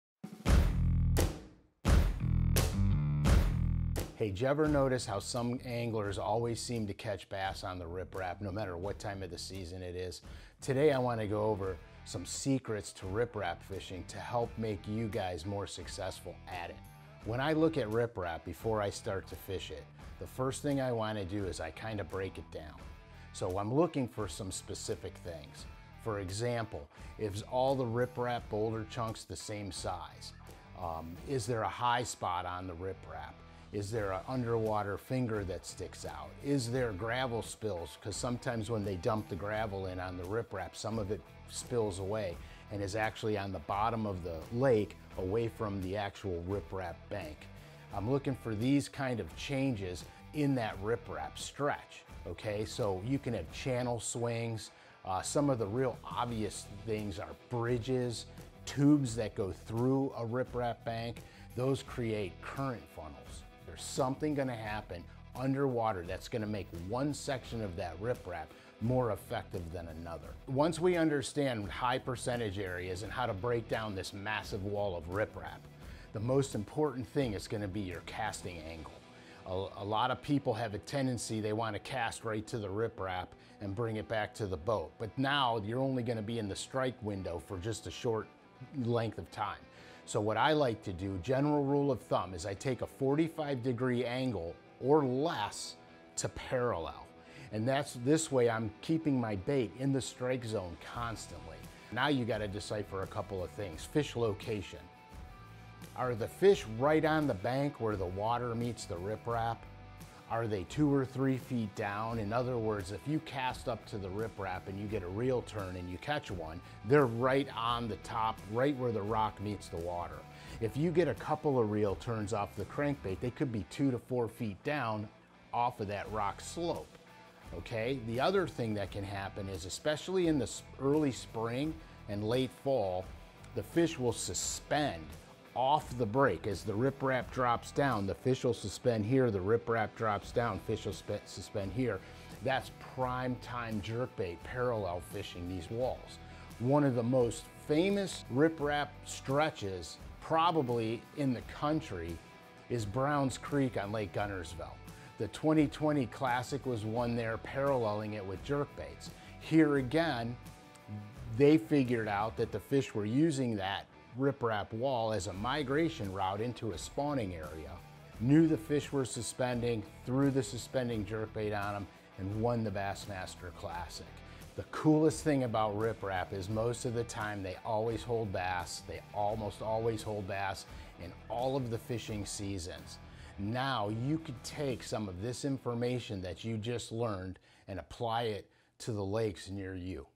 Why You Should Target Rip Rap for Year-Round Bass Fishing Success - Bait School
Want to learn where you can find a consistent bass bite during multiple seasons? Watch Frank Scalish as he deep dives into the topic of why you should target rip rap for year-round bass fishing success!